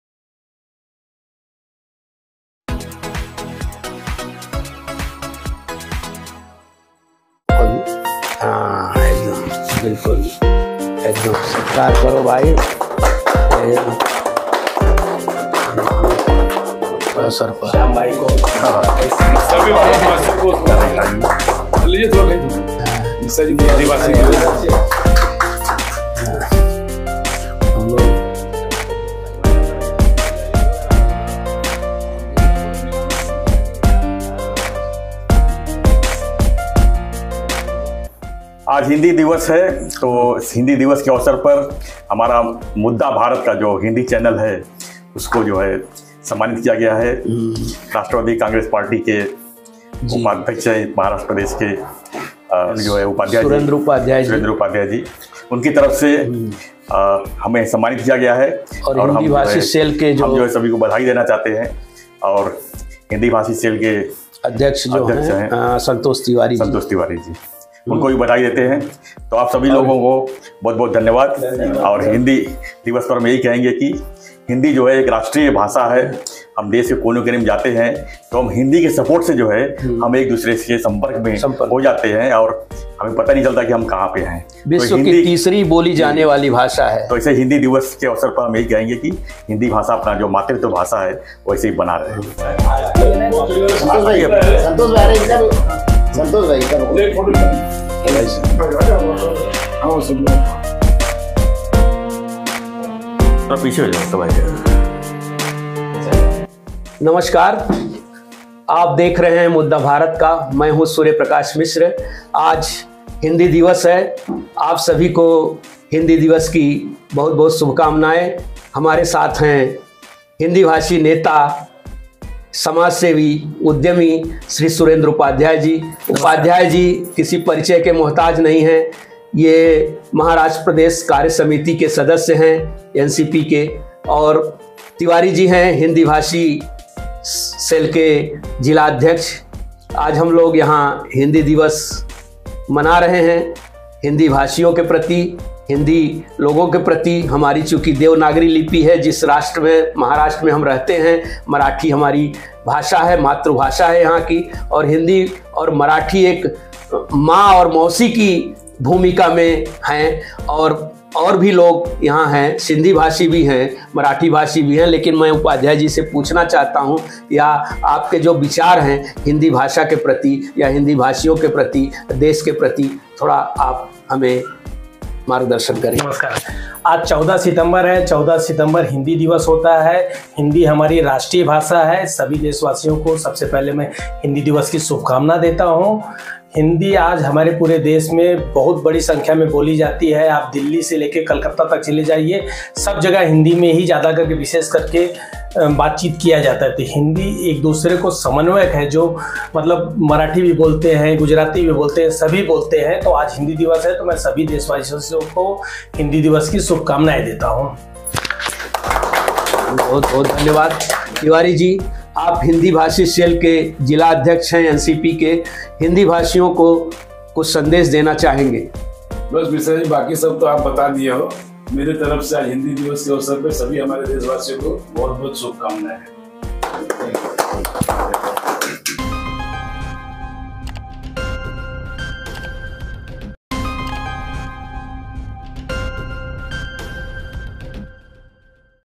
Hindi Divas | हिंदी दिवस की हार्दिक सुभकामनायें...Surendra Upadhyay, Santosh Tiwari, Suryaprakash
आज हिंदी दिवस है इस अवसर पर आज "मुद्दा भारत का" चैनल को सम्मानित किया गया एनसीपी शरद पवार गुट के उपाध्यक्ष सुरेन्द्र उपाध्याय व संतोष तिवारी साथ मे नवभारत के वरिष्ठ पत्रकार सूर्यप्रकाश मिश्रा उपस्थित थे...